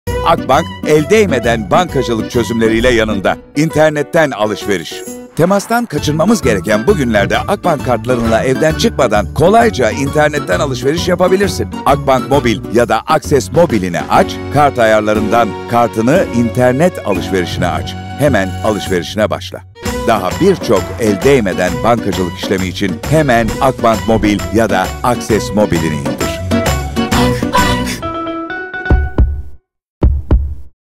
El Değmeden Bankacılık Çözümleri: İnternetten alışveriş
Sağlığın için evde kal, alışverişini internetten Akbank kartlarınla kolayca yap.